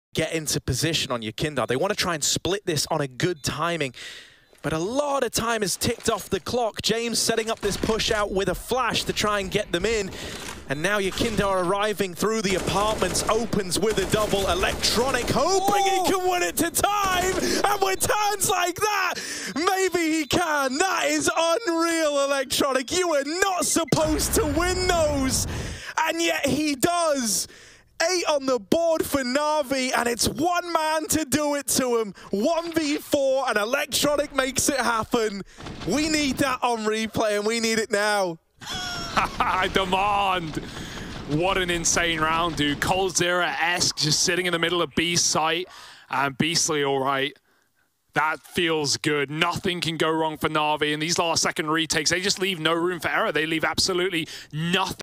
Incredible reaction from Electronic in 1vs4 clutch, NAVI vs VirtusPro, IEM Fall 2021 CIS
Electronic wins the hardest 1vs4 clutch for the T-side on Mirage